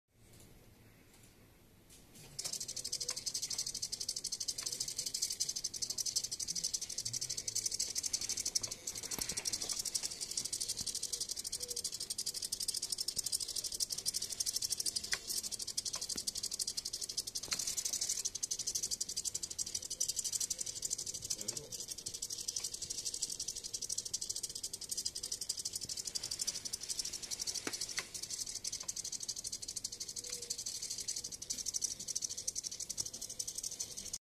Pololu Maestro and 4DBrix motors and monorail parts
4DBrix LEGO-compatible monorail parts controlled from my Ubuntu linux computer through Pololu Mini Maestro 24 channel Servo Controller.